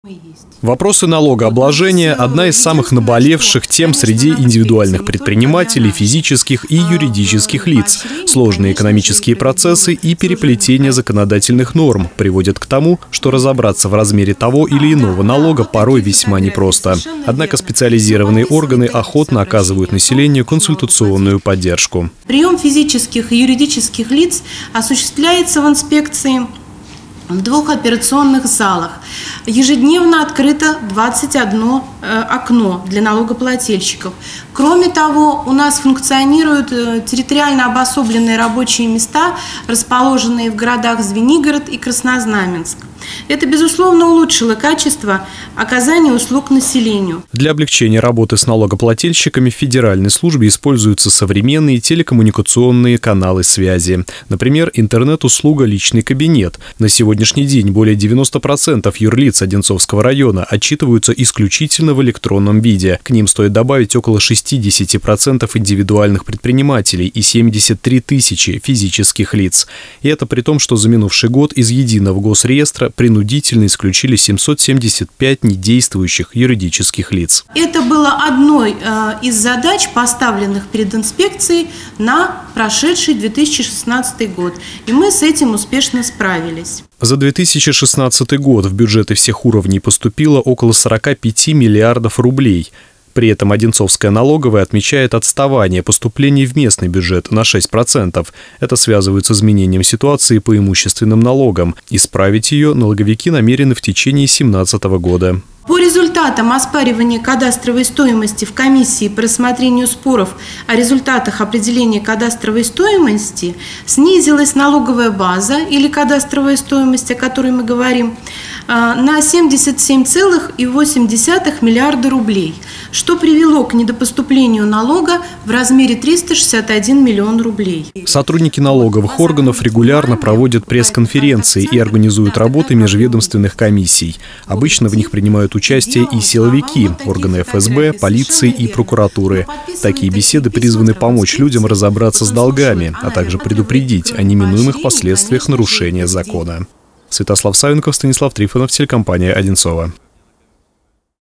Пресс конференция налоговой службы
Межрайонная Инспекция Федеральной налоговой службы России №22 по Московской области провела пресс-конференцию. Сотрудники службы озвучили некоторые итоги работы за 2016-ый год, а также обозначили планы на будущее.